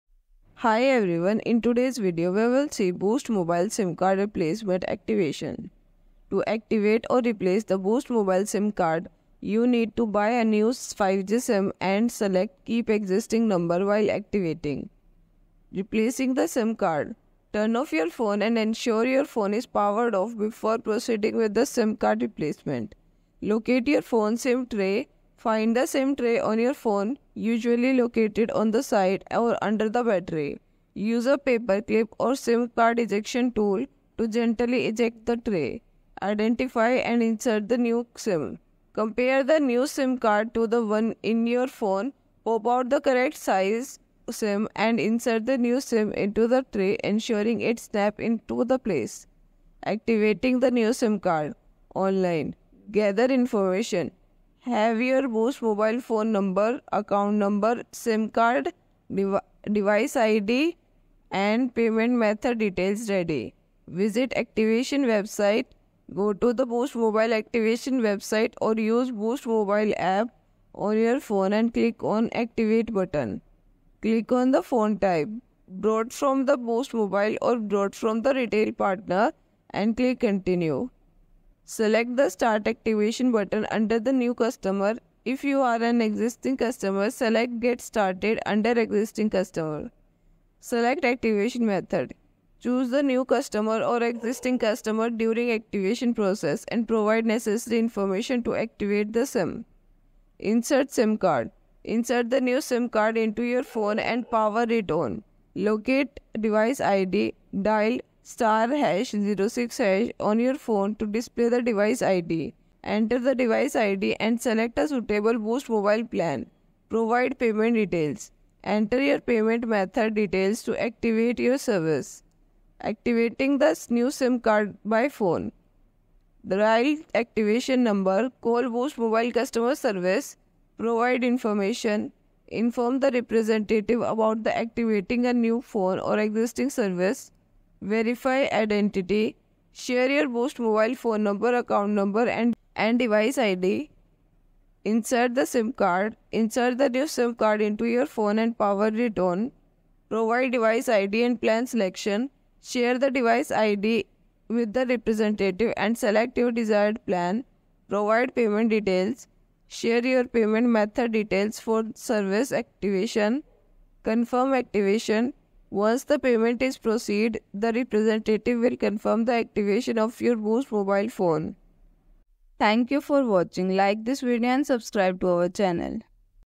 Boost Mobile Sim Card Replacement Activation
In this YouTube video, we will guide you through the process of replacing and activating a Boost Mobile SIM card. Whether you've lost your old SIM card or simply need a new one, we'll show you step-by-step how to get your new card up and running in no time.

00:00 Intro
00:15 Replacing The SIM Card
00:51 Activating The New SIM Card: Online
02:03 Activating The New SIM Card: Phone